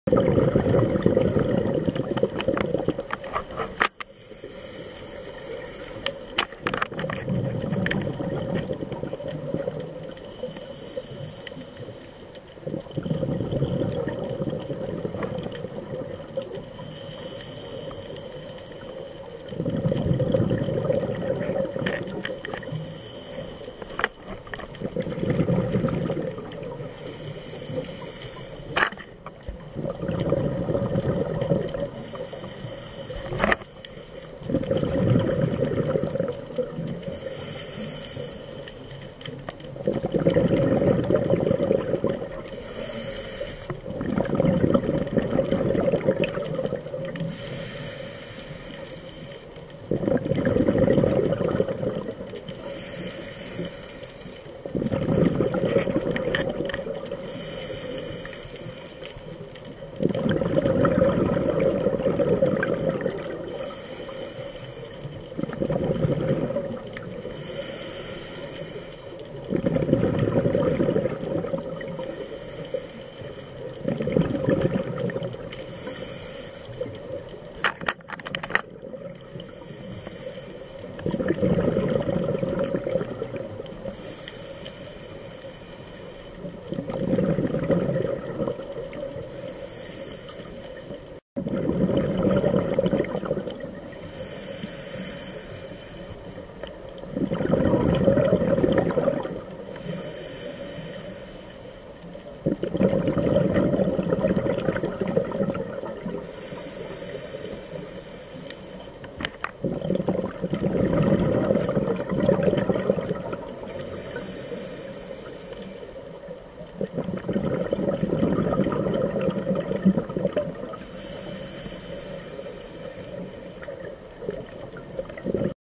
ダイビング動画「イラっとする顔のイラ(笑)」
2011年12月10日（土）熱海の冬季限定スポット「小曽我洞窟」。
洞窟をでた先でみたイラという魚。名前の語源は「イラっとする顔」だから「イラ」という名前が付いたらしい・・・。なんか嘘くさいな？カラフルな出で立ちで、顔をみても別にムカつかないぞ。かわいいじゃん。そう思いませんか？